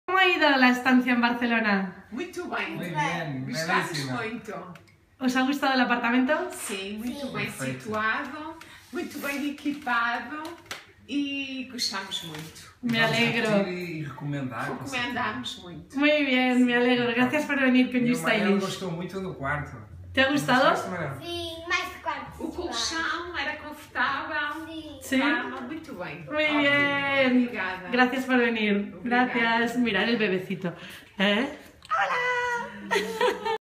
Adorable family that stayed with You Stylish Joao Estrada B244
This adorable family stayed in one of our two bedroom apartments. This is what they had to say about it.